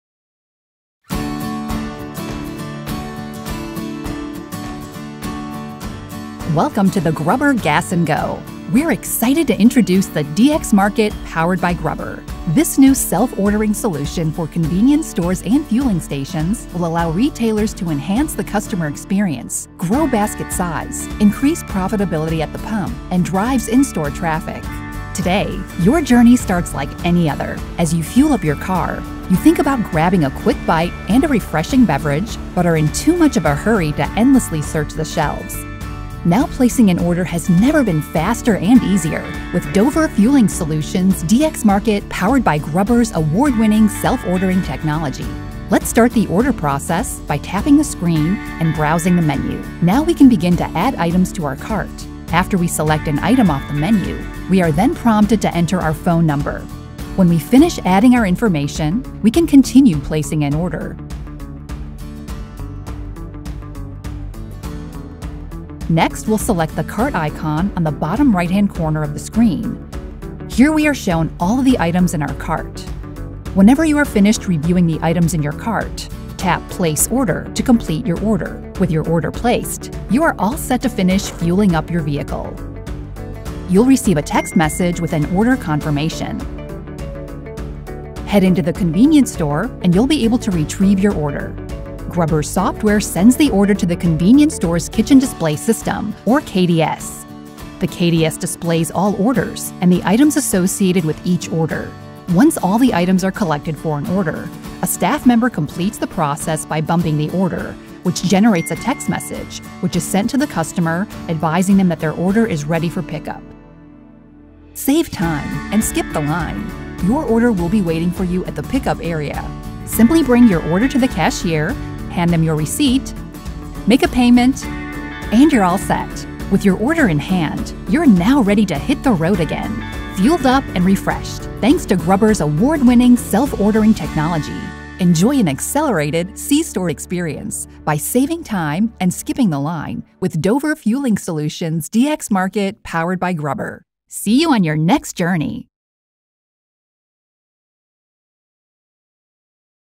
DX Market™ powered by GRUBBRR®
DFS and GRUBBRR have joined forces to revolutionize the customer experience at the pump.

Introducing DX Market™: This new self-ordering solution for convenience stores and fueling stations will allow retailers to enhance customer experience, grow basket size and increase profitability at the pump.